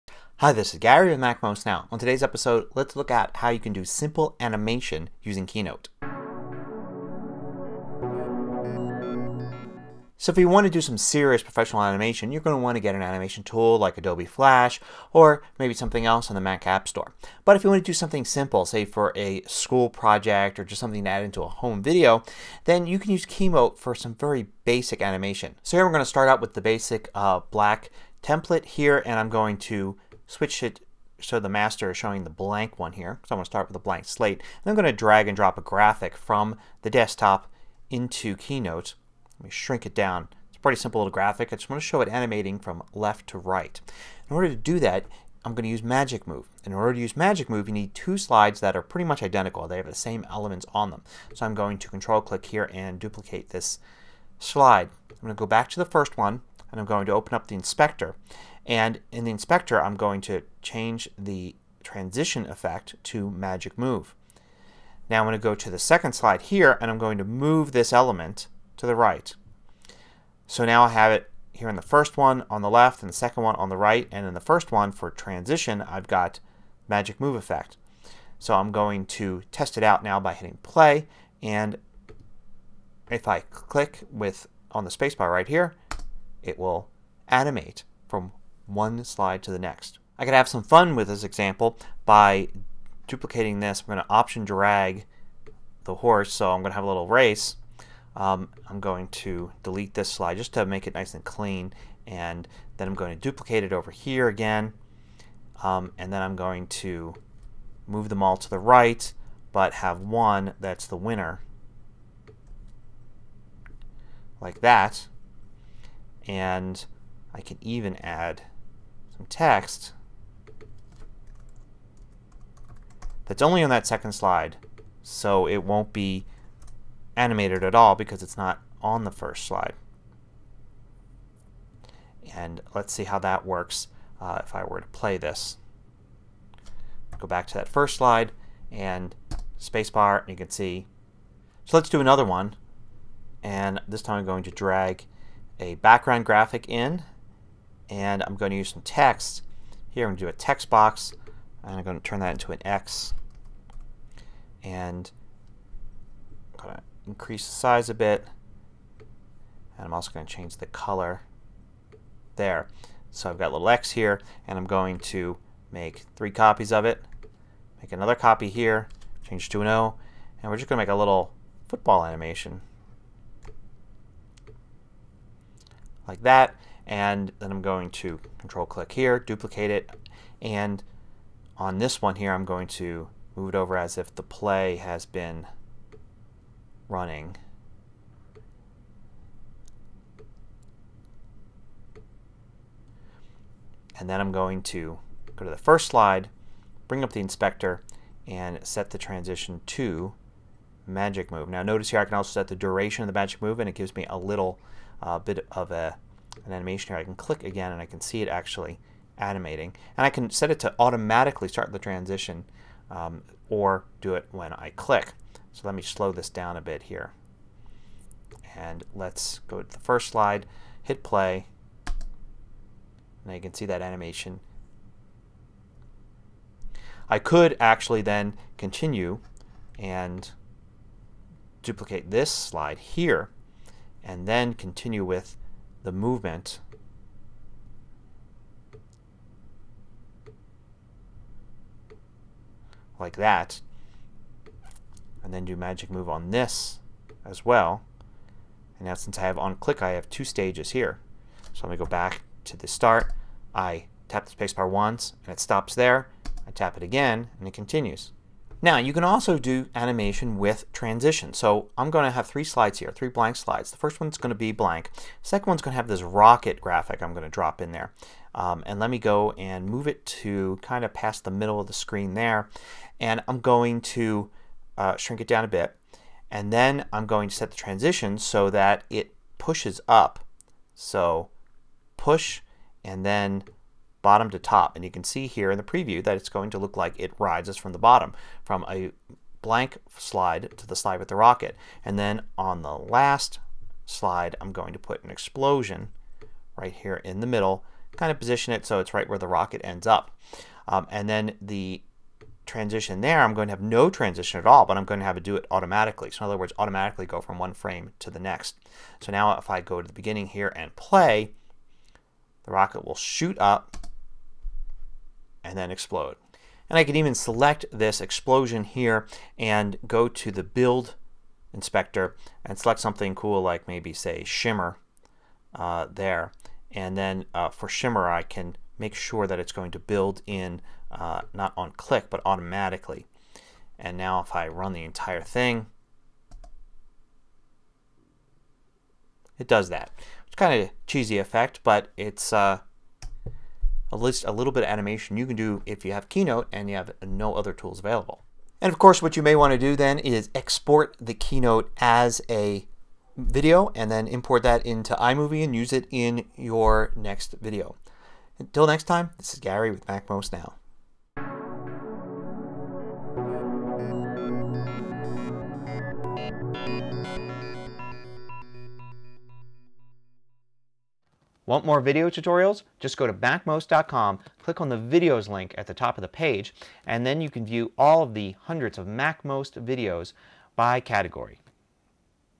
Simple Animation With Keynote (MacMost Now 809)
Keynote can be used to create simple animation. You can use the Magic Move transition to animate objects and move them to different positions on the screen. You can also use transitions to create simple movement and special effects. You can use these in your Keynote presentations, or export them to video file to use in iMovie.